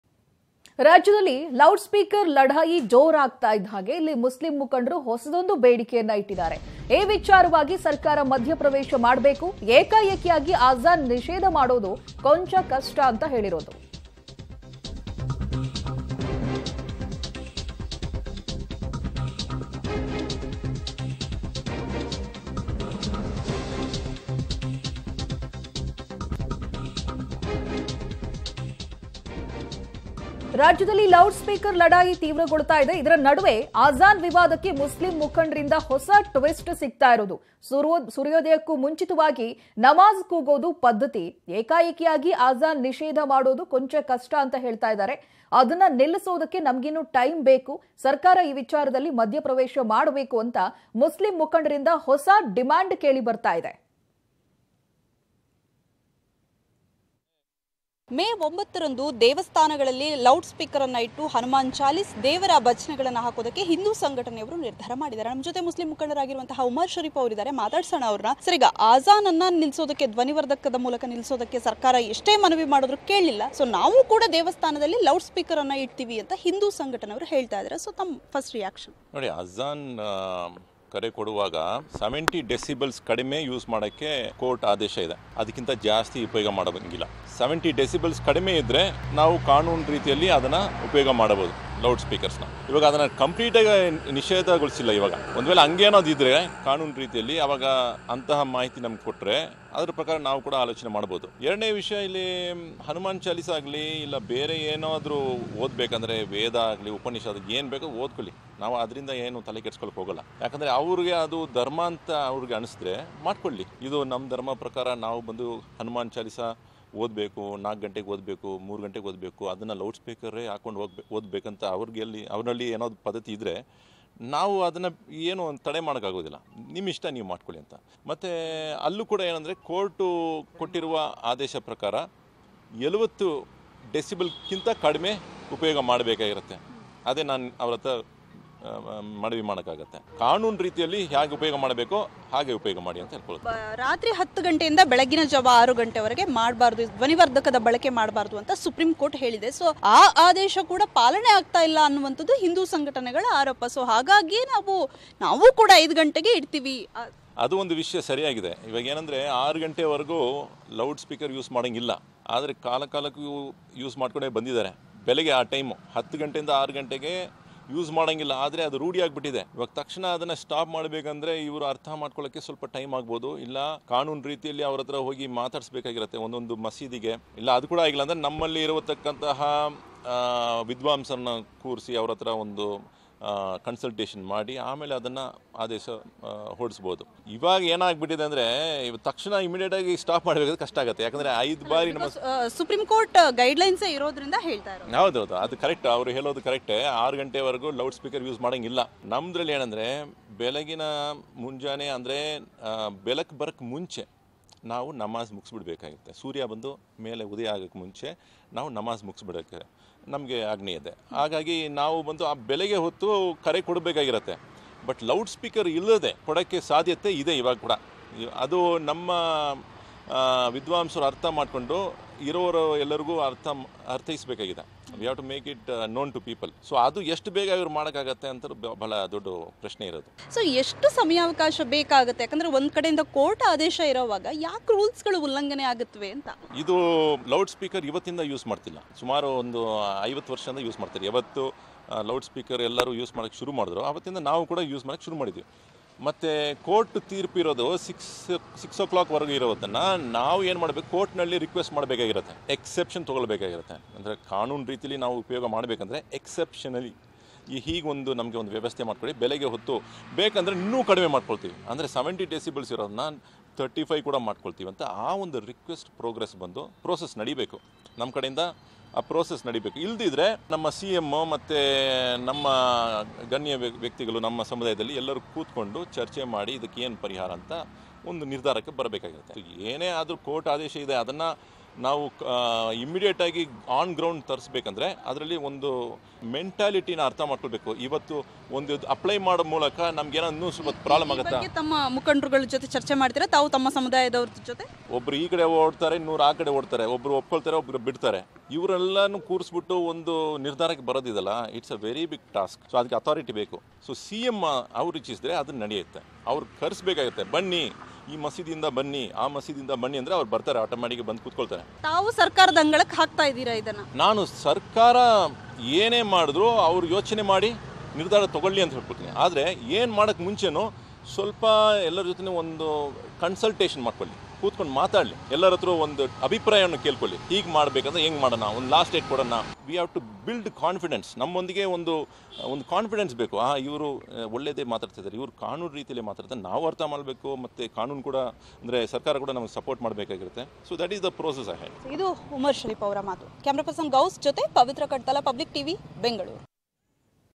Muslim Leader Umar Shariff Says Government Should Intervene In 'Loudspeaker' Fight
ರಾಜ್ಯದಲ್ಲಿ ತೀವ್ರಗೊಂಡ ಲೌಡ್ ಸ್ಪೀಕರ್ ಲಡಾಯಿ..!
ಆಜಾನ್ ವಿವಾದಕ್ಕೆ ಮುಸ್ಲಿಂ ಮುಖಂಡರಿಂದ ಹೊಸ ಟಿಸ್ಟ್..!
ಸೂರ್ಯೋದಯಕ್ಕೂ ಮುನ್ನ ನಮಾಜ್ ಕೂಗೋದು ಪದ್ಧತಿ
ಏಕಾಏಕಿ ಆಜಾನ್ ನಿಷೇಧ ಮಾಡೋದು ಕೊಂಚ ಕಷ್ಟ..!
`ಅದನ್ನು ನಿಲ್ಲಿಸಲು ನಮಗಿನ್ನೂ ಟೈಮ್ ಬೈಕು..'
ಸರ್ಕಾರ ಈ ವಿಚಾರದಲ್ಲಿ ಮಧ್ಯ ಪ್ರವೇಶ ಮಾಡಬೇಕು...
ಮುಸ್ಲಿಂ ಮುಖಂಡರಿಂದ ಹೊಸ ಡಿಮ್ಯಾಂಡ್..!